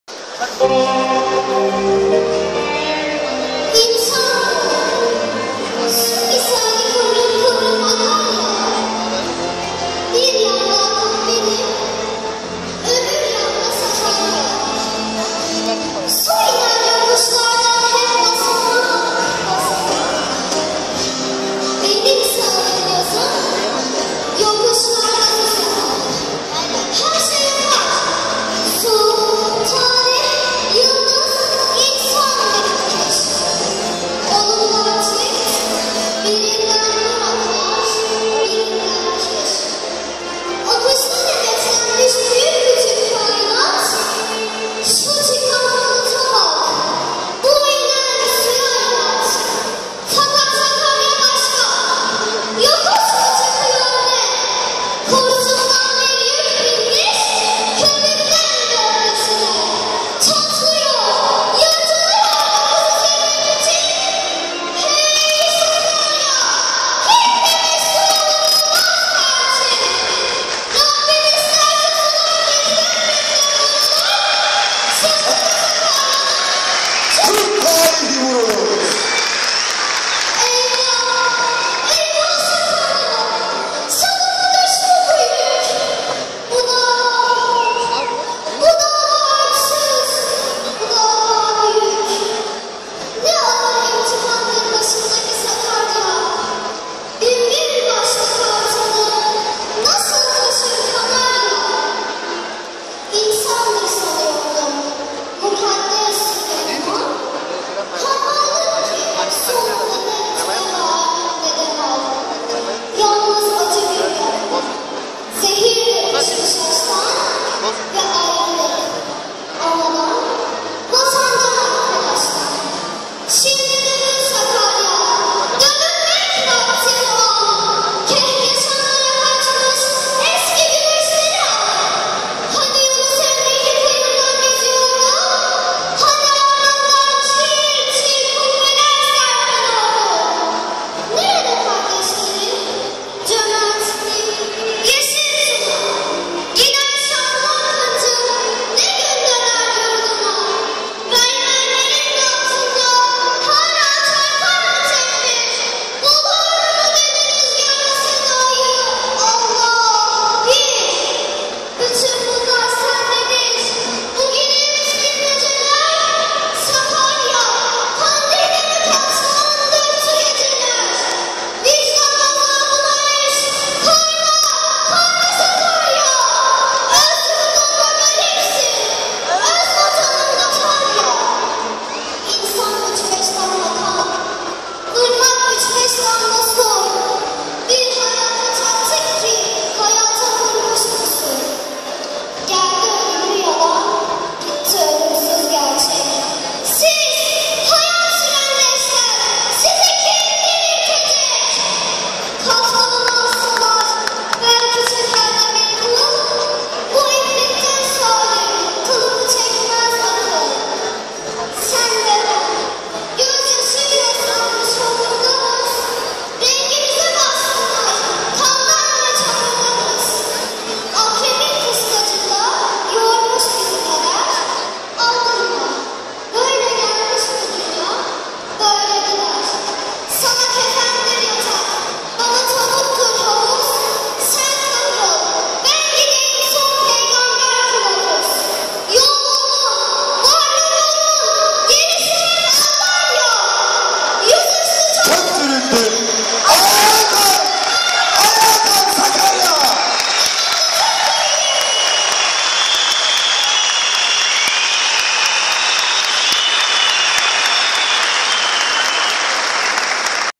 NİSA SUDE KESKİN SAKARYA ŞİİRİ
Bursa Ülkü Ocaklarının düzenlediği ''SİZ GELECEKSİNİZ,GELECEK SİZİN'' adlı Ahmet Şafak-Osman Öztunç konseri.